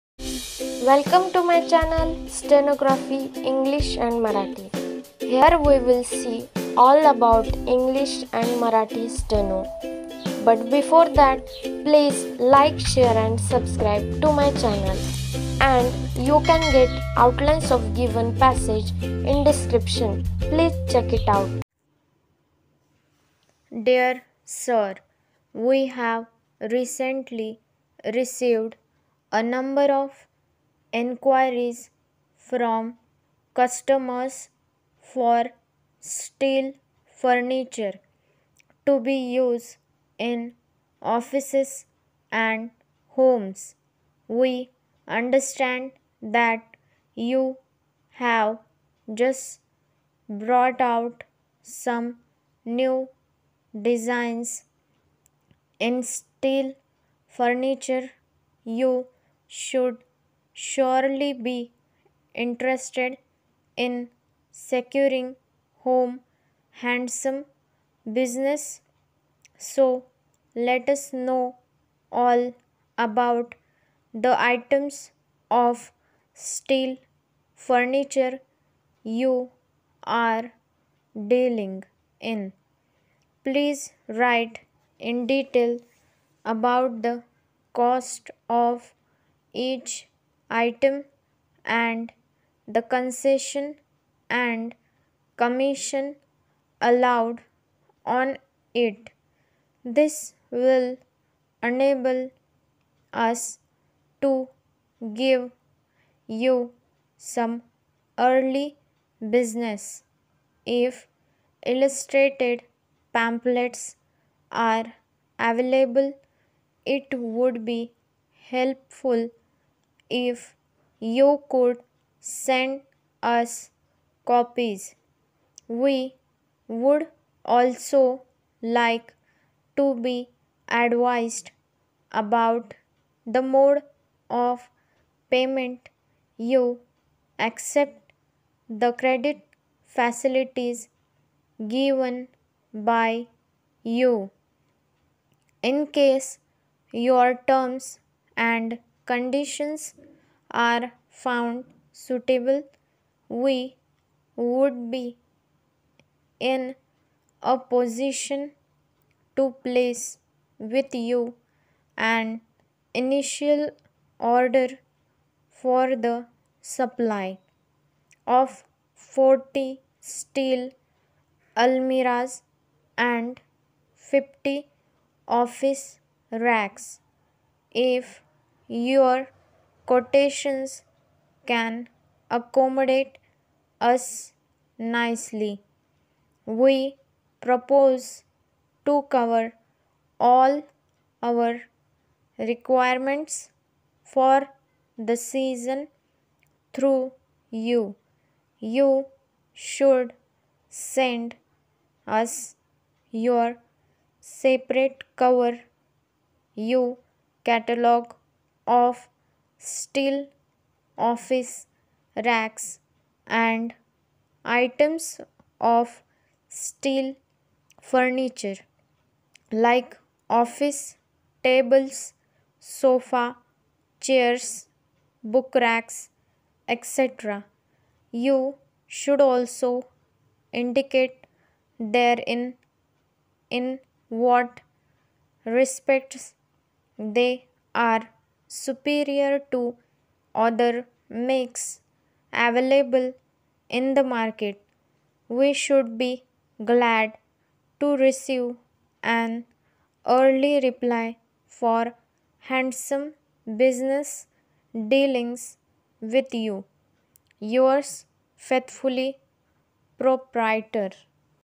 English Shorthand 60 WPM (B) GCC - TBC Question Paper 28 Oct, 2021 | Last Year GCC- TBC Q.P
@ stenography english and marathi

 ENGLISH SHORTHAND 60 WPM DICTATION .

Hiii
Friends,

About this video :
      In given video I have given 60 WPM paragraph of english shorthand . do practice with it and increase your speed of stenography writing .

* PLAYLIST OF GCC - TBC  MARATHI SHORTHAND QUESTION PAPERS

*PLAYLIST OF GCC - TBC ENGLISH SHORTHAND QUESTION PAPERS